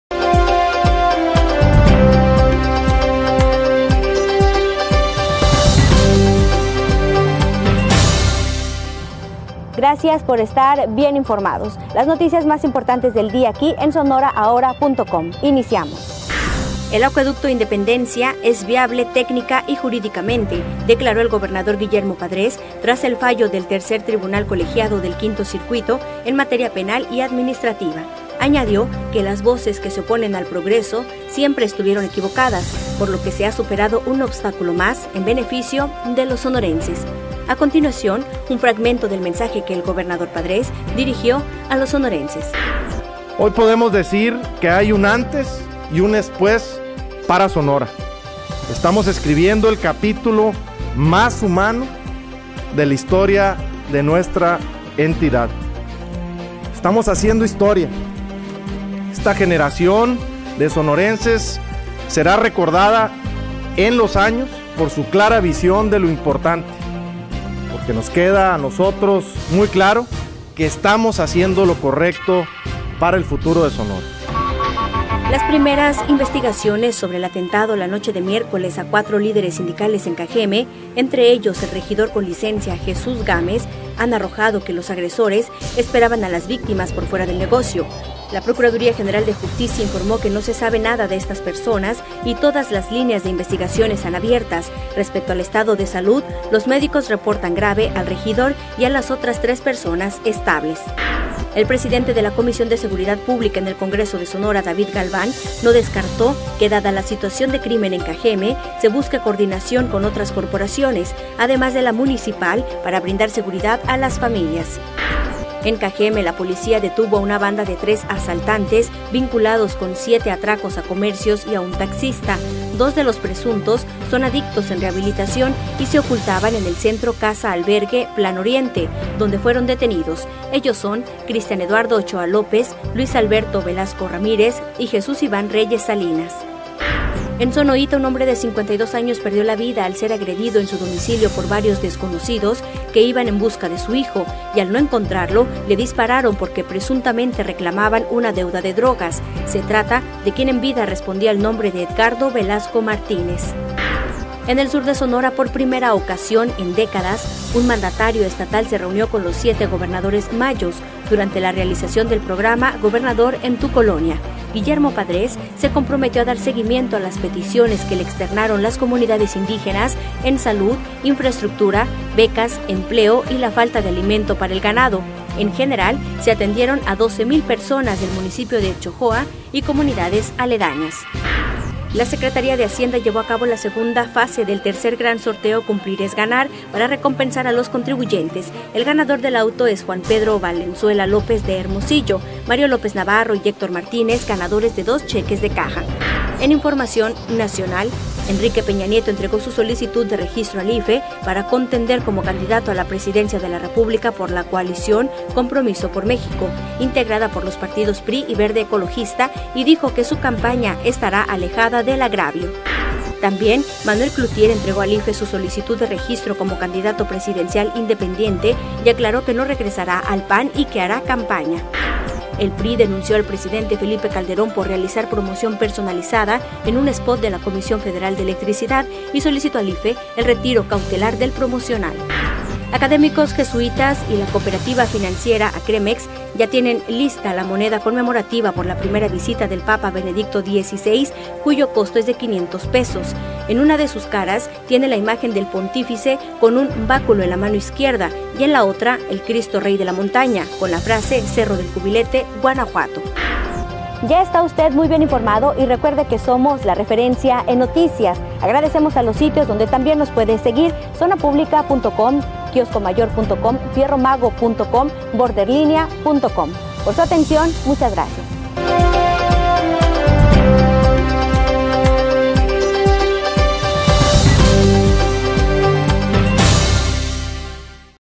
El noticiario sonoraahoratv 16032012.mp4
EL ACUEDUCTO SIGUE Y NO SE DETIENE

El Acueducto Independencia es viable técnica y jurídicamente, declaró el gobernador Guillermo Padrés tras el fallo del Tercer Tribunal Colegiado del Quinto Circuito en Materia Penal y Administrativa. Añadió que las voces que se oponen al progreso "siempre estuvieron equivocadas" por lo que se ha superado un obstáculo más en beneficio de los sonorenses.

A continuación el mensaje completo que el Gobernador Padres dirigió a los sonorenses.

INVESTIGAN ATENTADO EN CAJEME

Las primeras investigaciones sobre el atentado la noche de miércoles a cuatro líderes sindicales en Cajeme, entre ellos el regidor con licencia Jesús Gámez han arrojado que los agresores esperaban a las víctimas por fuera del negocio, la Procuraduría General de Justicia informo que no se sabe nada de los agresores y todas las líneas de investigación están abiertas. Respecto al estado de salud, los médicos reportan grave al regidor y a las otras 3 personas estables.

PEDIRAN APOYO POR VIOLENCIA

El presidente de la Comisión de Seguridad Pública en el Congreso de Sonora David Galván no descartó que dada la situación de crimen en Cajeme, se busque coordinación con otras corporaciones, además de la Municipal para brindar seguridad a las familias.

ATRAPAN  BANDA DE ASALTANTES

En Cajeme la policía detuvo a una banda de 3 asaltantes vinculados con 7 atracos a comercios y un taxista. Dos de los presuntos, son adictos en rehabilitación, y se ocultaban en el centro Casa Albergue Plano Oriente donde fueron detenidos, ellos son, Cristian Eduardo Ochoa López, Luis Alberto Velasco Ramírez  y Jesús Iván Reyes Salinas.

PAGAN JUSTOS POR PECADORES

En Sonoyta un hombre de 52 años perdió la vida al ser agredido en su domicilio por varios desconocidos que iban en busca de su hijo y al no encontrarlo le dispararon porque presuntamente reclamaban una deuda de drogas. Se trata de quien en vida respondía al nombre de Edgardo Velasco Martínez.

GOBERNADOR EN TU COLONIA

En el sur de Sonora por primera ocasión en décadas un mandatario estatal se reunió con los siete Gobernadores Mayos, durante la realización del programa Gobernador en tu Colonia. Guillermo Padres se comprometió a dar seguimiento a las peticiones que le externaron las comunidades indígenas en salud, infraestructura, becas, empleo y la falta de alimento para el ganado. En general se atendieron a 12 mil personas del municipio de Etchojoa y comunidades aledañas
PREMIAN A CONTRIBUYENTES CUMPLIDOS

La secretaria de hacienda llevo a cabo la segunda fase del Tercer Gran Sorteo, "Cumplir es Ganar" para recompensar a los contribuyentes, el ganador del auto es Juan Pedro Valenzuela López de Hermosillo, Mario López Navarro y Héctor Martínez ganadores de 2 cheques de caja.

EN INFORMACION NACIONAL.

VIOLENCIA AL TOPE

El gobernador de Coahuila, Rubén Moreira Valdez, demandó mayor presencia de fuerzas militares y federales para reforzar la seguridad pues las agresiones de los ultimos dias han llevado a la entidad "hasta el tope" de la inseguridad e intranquilidad ciudadana, señalo.

REGISTRA CANDIDATURA PEÑA NIETO

Enrique Peña Nieto entregó su solicitud de registro al (IFE) para contender como candidato a la Presidencia de la República por la coalición "Compromiso por México" integrada por los partidos (PRI) y Verde Ecologista  y dijo que su campaña estará "alejada del agravio".

REGISTRO CLOUTIER

También Manuel Clouthier entregó al (IFE) su solicitud de registro como candidato presidencial independiente y aclaró que no regresara al (PAN) y que hará campaña.

PRI ACUSA A CALDERON

El PRI denunció al presidente Felipe Calderón por realizar promoción personalizada en un spot de la Comisión Federal de Electricidad y solicitó al IFE el retiro cautelar del promocional.

MONEDA CONMEMORATIVA DEL PAPA

Académicos jesuitas y la Cooperativa Financiera ACREMEX ya tienen lista la moneda conmemorativa por la primera visita del Papa Benedicto XVI cuyo costo es de 500 pesos. En una de sus caras tiene la imagen del pontífice con un báculo en la mano izquierda y en la otra el Cristo Rey de la Montaña con la frase "Cerro del Cubilete, Guanajuato".